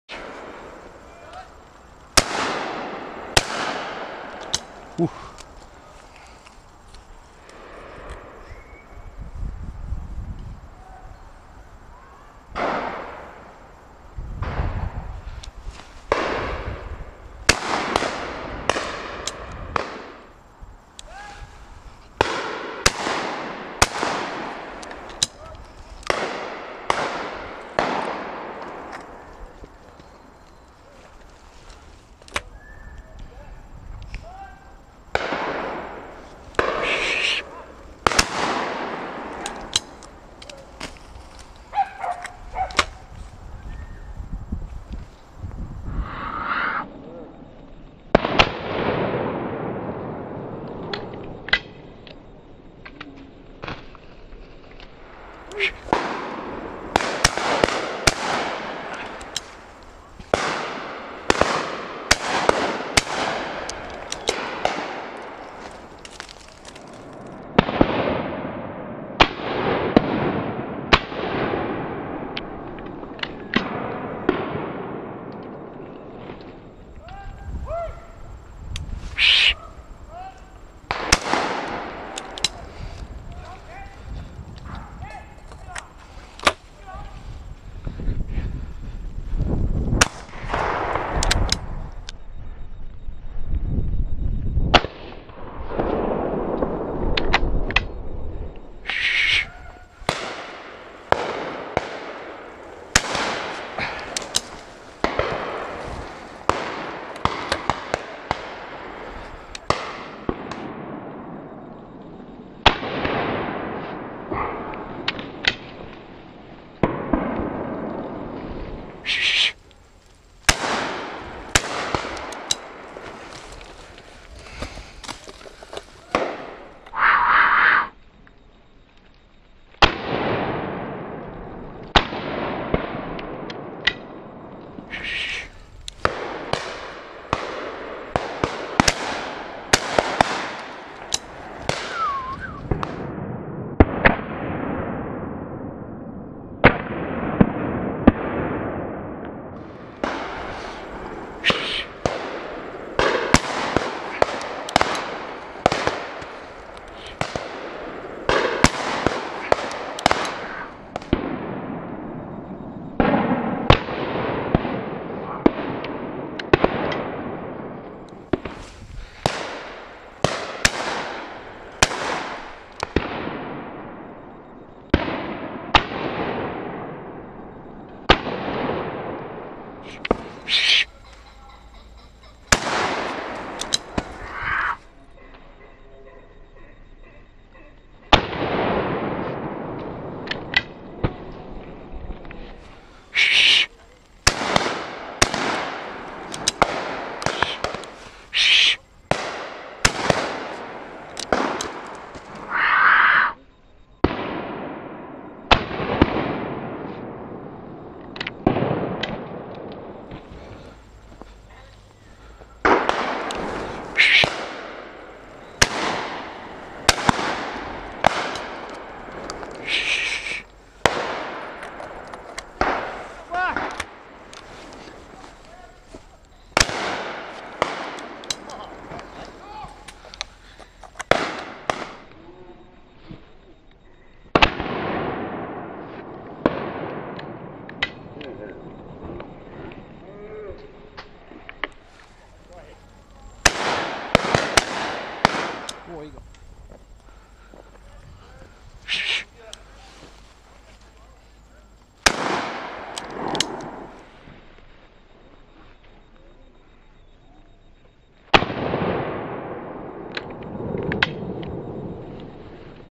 Driven Pheasant Shooting - Long Mountain Shoot - Beaters Day 2019
Beaters day at The Old Long Mountain shoot near Welshpool